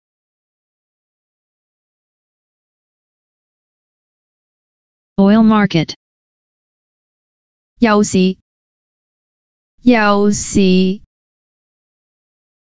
"OIL MARKET" in Cantonese (油市) - Flashcard
[yau4 si5] "OIL MARKET" in Cantonese (油市) - Flashcard. Additional information about the word: oil market.